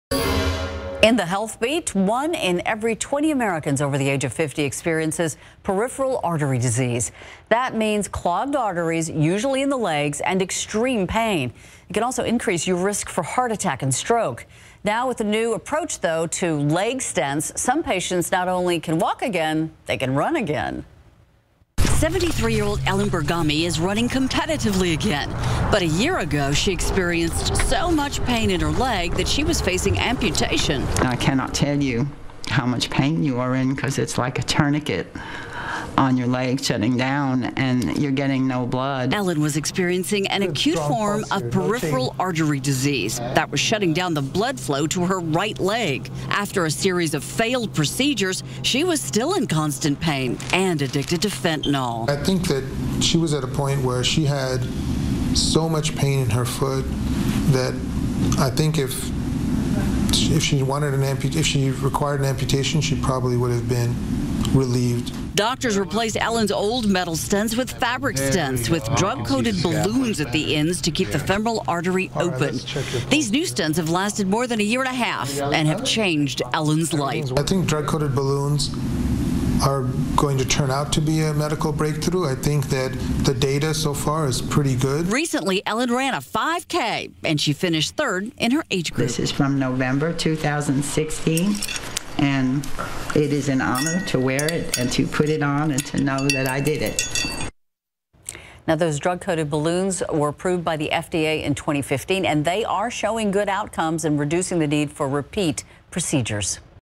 Video: New leg stent gets patient back in the race
A new approach to leg stents has some patients not only walking, but also running.